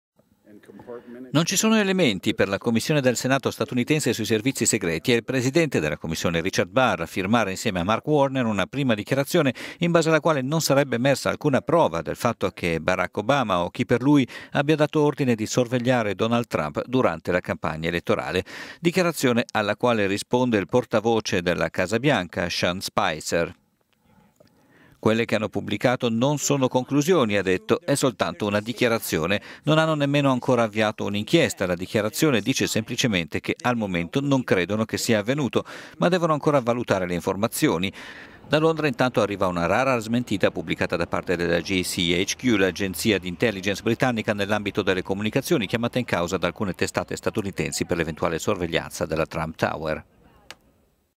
USA: sospetto spionaggio a Trump, Senato non ci crede
"Non ci sono elementi", per la Commissione del Senato statunitense sui servizi segreti: è il presidente della commissione, Richard Burr, a firmare insieme a Mark Warner una prima dicharazione in base alla quale non sarebbe emersa alcuna prova del fatto che Barack Obama o chi per lui abbia dato ordine di sorvegliare Donald Trump durante la campagna elettorale.

Dichiarazione alla quale risponde il portavoce della Casa Bianca, Sean Spicer:
"Quelle che hanno pubblicato non sono conclusioni, è sol…